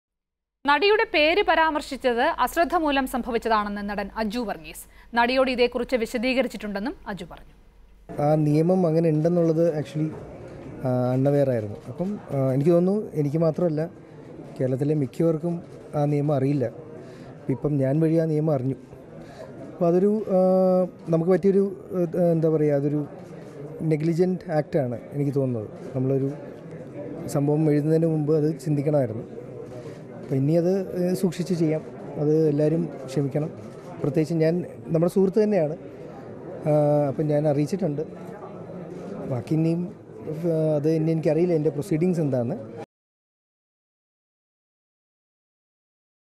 Assaulted actress' name mentioning was happened due to negligence; says Aju Varghese
Assaulted actress' name mentioning was happened due to negligence, has explained about the same to actress says Aju Varghese.